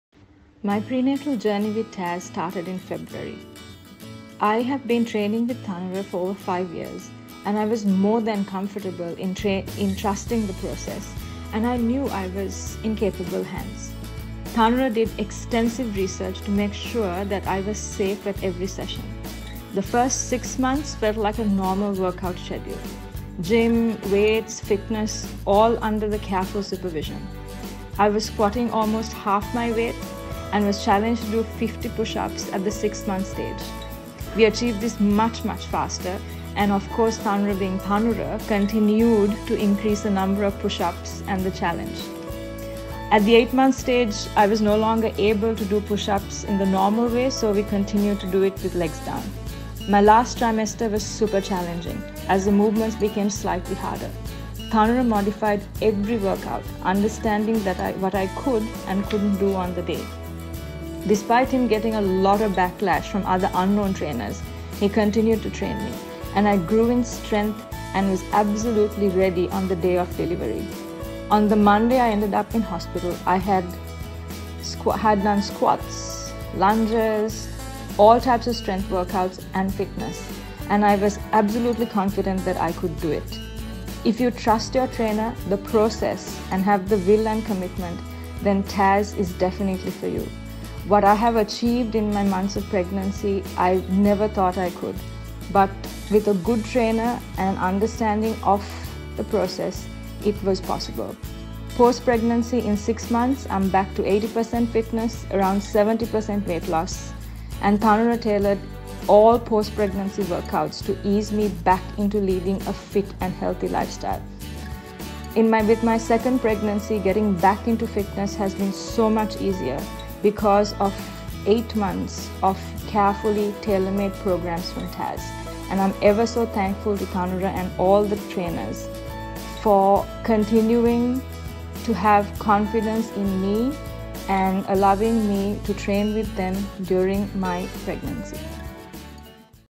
Dharshi Speaks on TASS (Pre & Post Natal Exercise)
Dharshi has been with TASS for over 5 years, so the training conducted during her pregnancy was based on the 5 years of experience in training her and understanding her limitations. Therefore don't be too alarmed about some of the exercises she does (specially the balancing exercises) we were confident that the exercises could be performed with ease and without any risk to her or the baby.